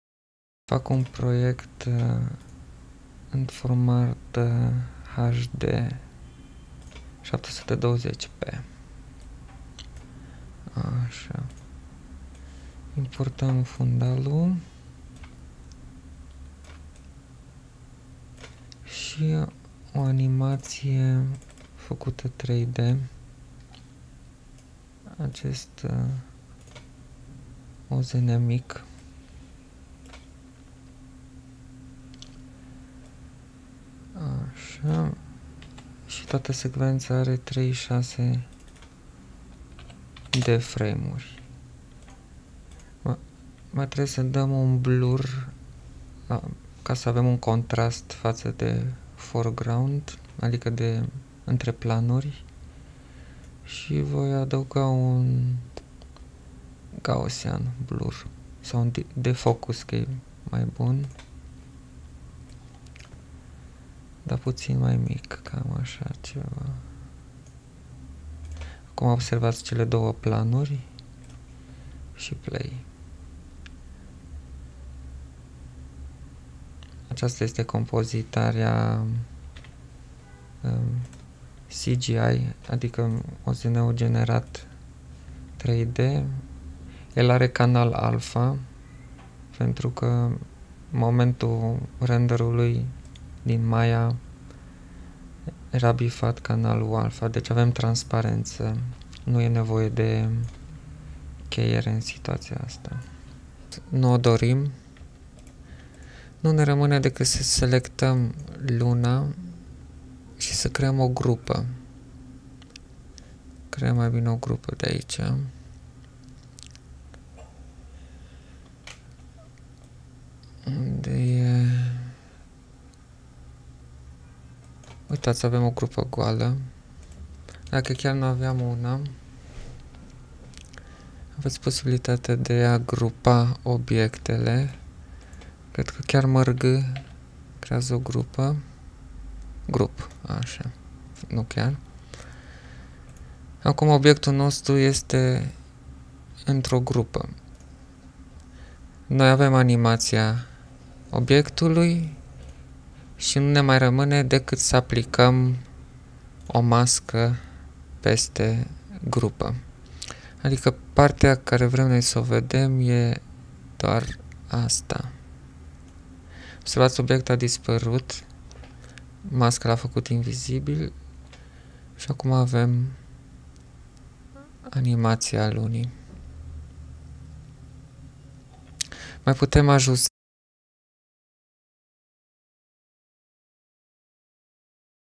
itraining.ro - FCS101 - Final Cut Studio 2
introducere in elementele esentiale ale pachetului Final Cut Studio 2 (Apple MAc - OSX).